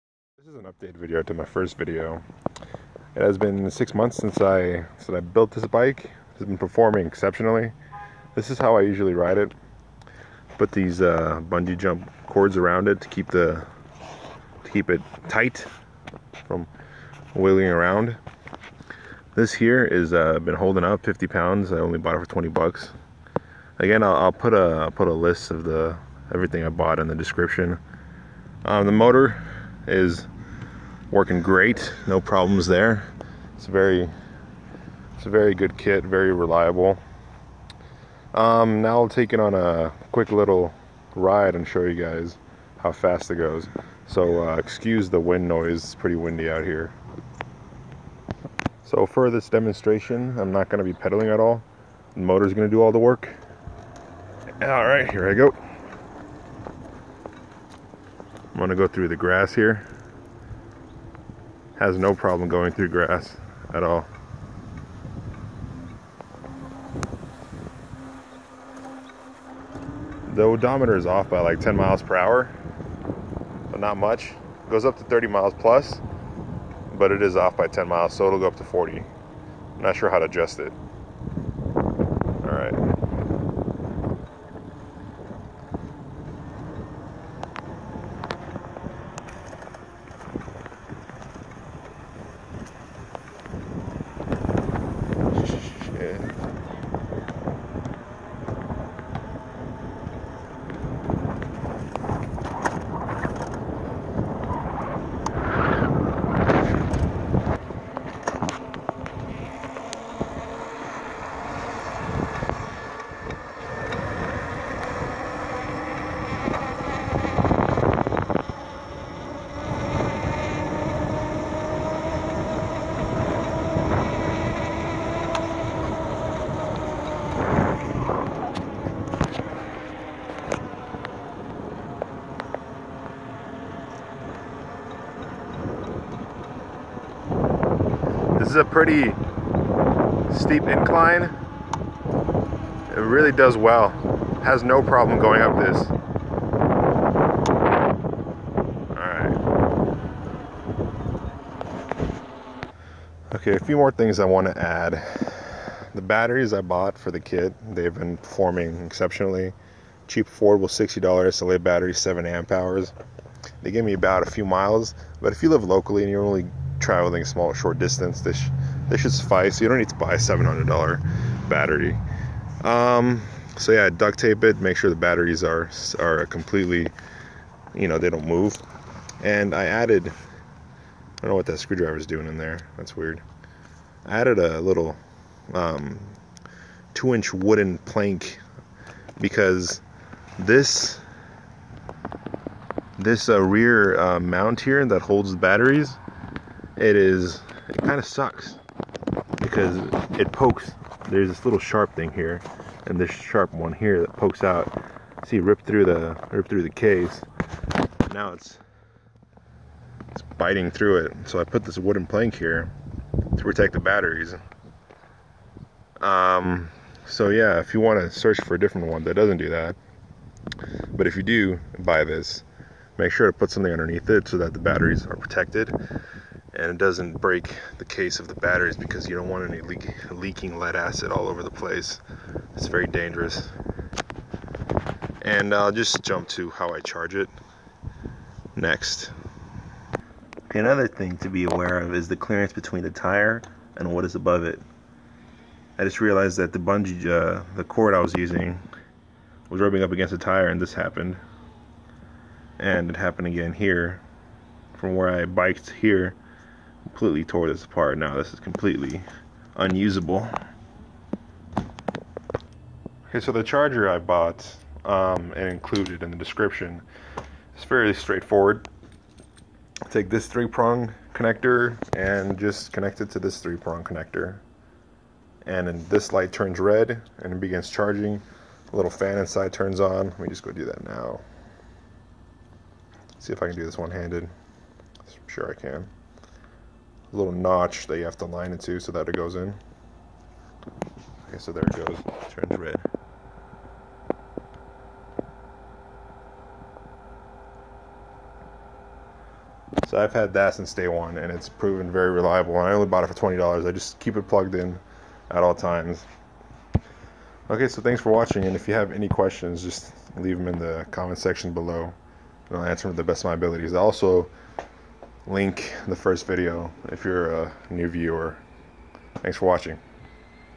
Amazing and Affordable Electric Bike! Part 2
Click the link for the Part 1 of this video

Total Cost for just the motor and parts: A little over $400.

Here is a list of everything that I bought and used for this project:

Bike Rear Wheel Electric Motor:

Lead Acid Battery 7aH:

48V Volt battery Charger:

3 Prong Charging Jack:

Pedro's Bicycle Tire Lever:

Zoomable Flashlight with Mount Holder :

12 Gauge Wire:

Female Disconnect:

Bullet Splice:

Recommended things but not required:

-Multi-meter for measuring voltage

-Wire Stripper

-Crimping Tool

-Duct Tape

-Electrical Tape

Few things I forgot to mention in the video:
The reason I almost electrocuted myself was because I connected the batteries incorrectly. Differently than the diagram. So if you follow the diagram everything will go smoothly.